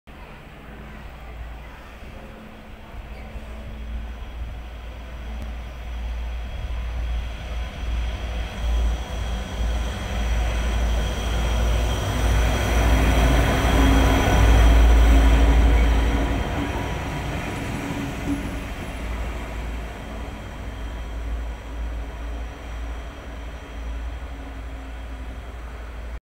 Dúo de 334 aisladas por Villaverde Bajo
Un par de locomotoras Renfe 334 circulan aisladas por Villaverde Bajo (334-016+334-025)